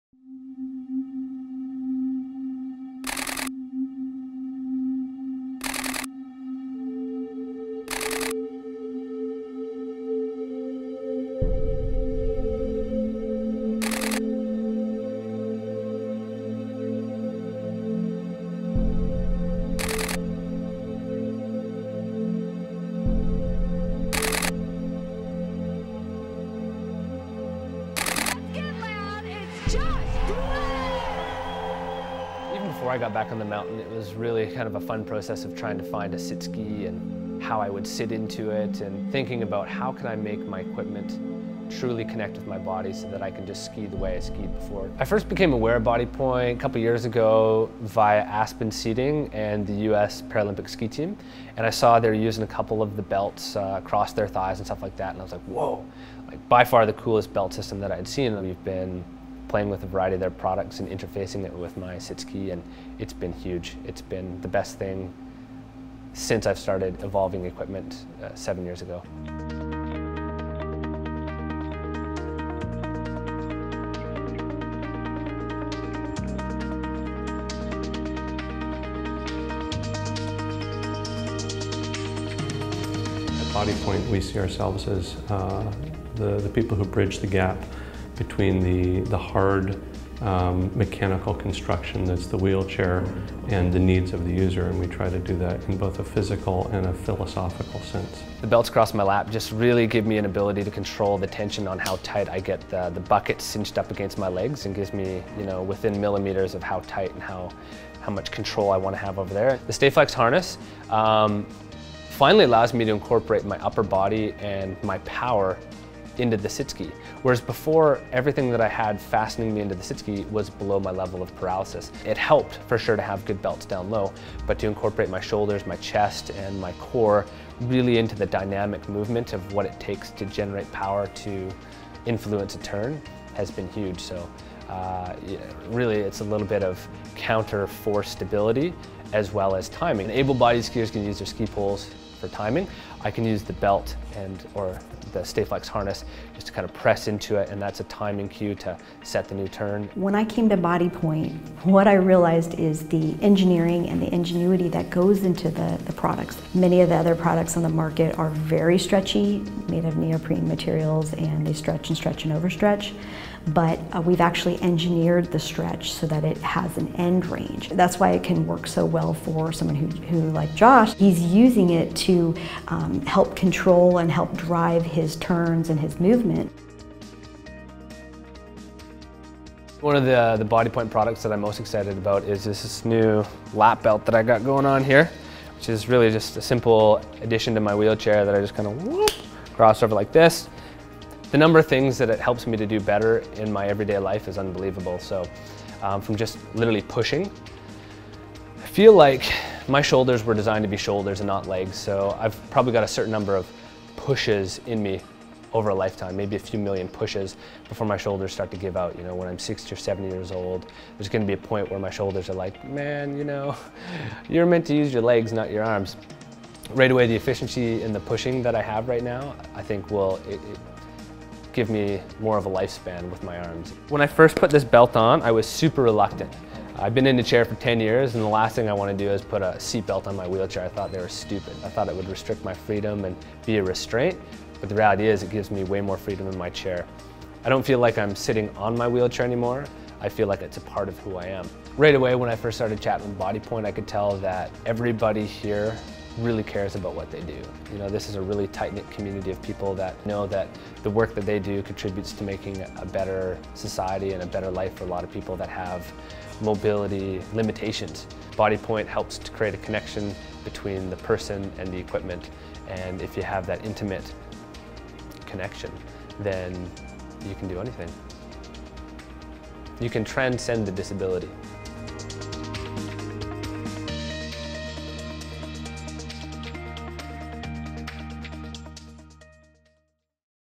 Bodypoint | Josh Dueck: Wheelchair Positioning Transcends Disability
At 23 years old, as coach of the Silver Star Freestyle Ski Club in Vernon, B.C. Josh Dueck dislocated his back and severed his spinal cord doing a demonstration jump for his students. Nine months after his fall, he was back on a ski hill riding a sit ski…Now a world champion, and Paralympic Gold Medalist.
Before he got back on the mountain he did a lot of thinking about how he could make his equipment truly connect with his body so that he could ski the way he skied before. This is his story about Bodypoint products and how they have helped transcend his disability using proper positioning.